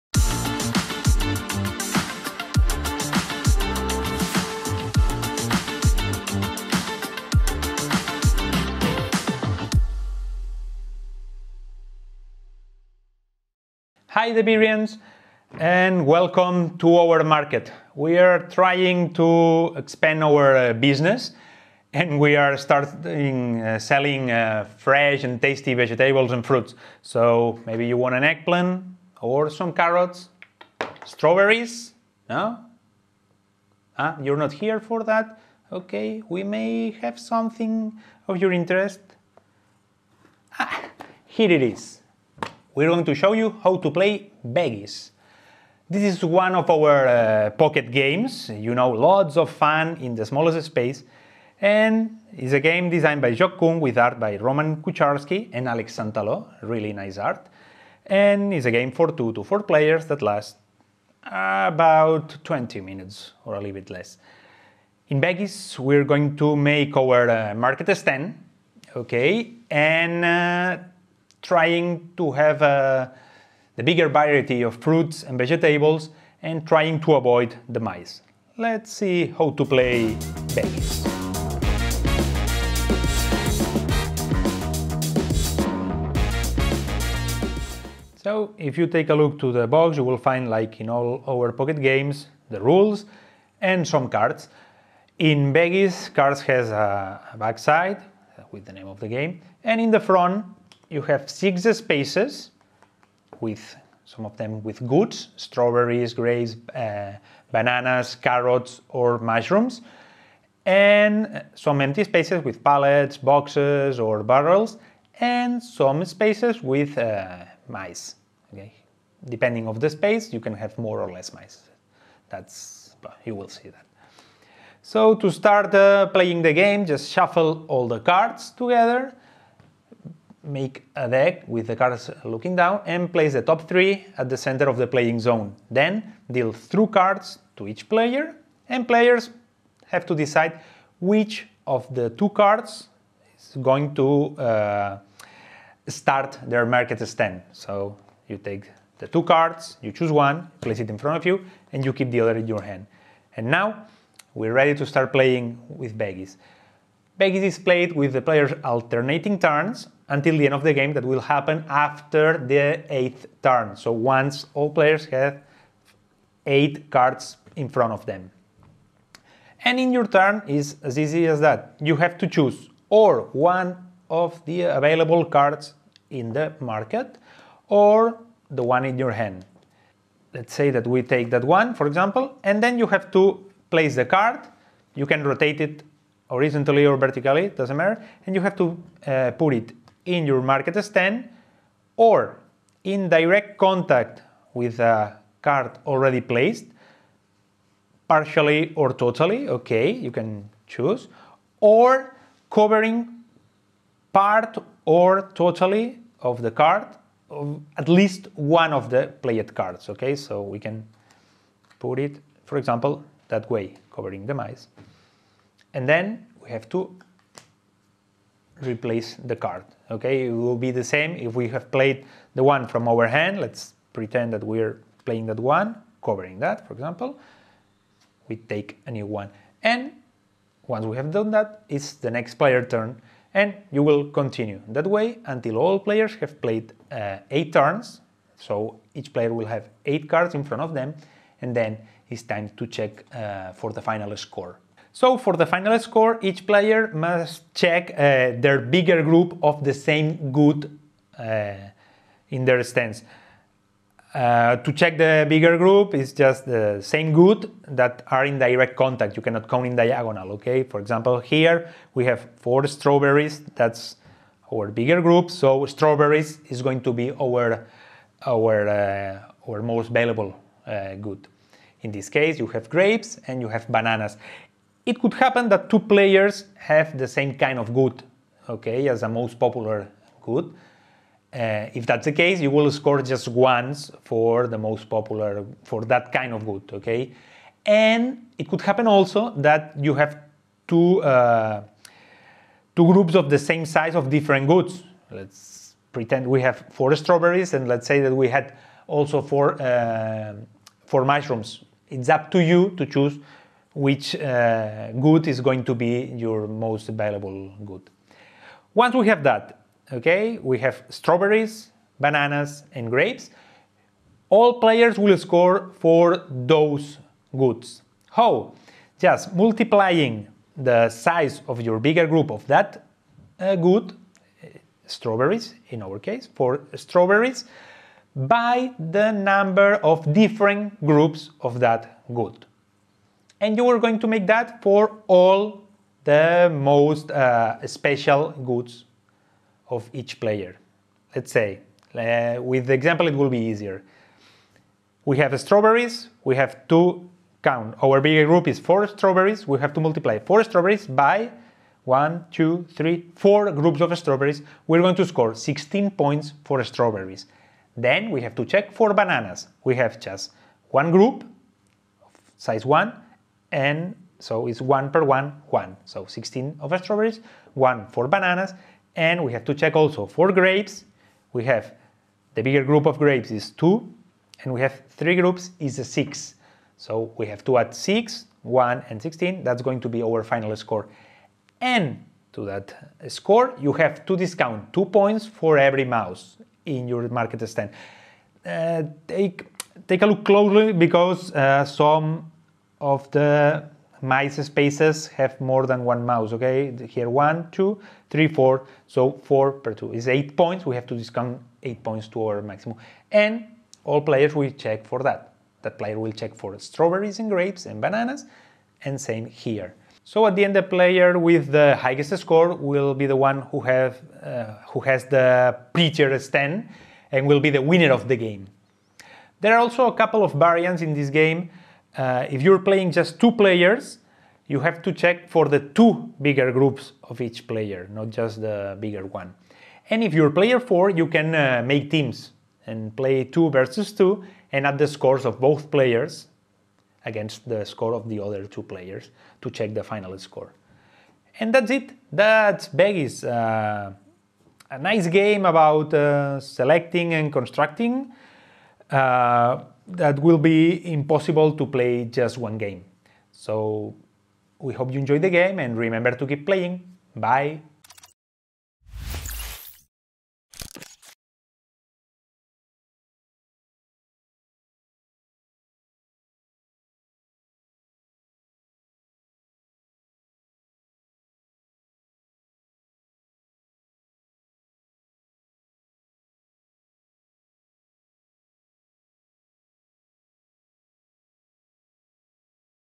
How To Play Veggies
Fresh fruit and veggies! The best in the market! Look at these strawberries, ma’am!

In Veggies, 2 to 4 players compete to build the most successful stand in the market. They’ll organize their produce stands in the best way possible to earn the most victory points in the game.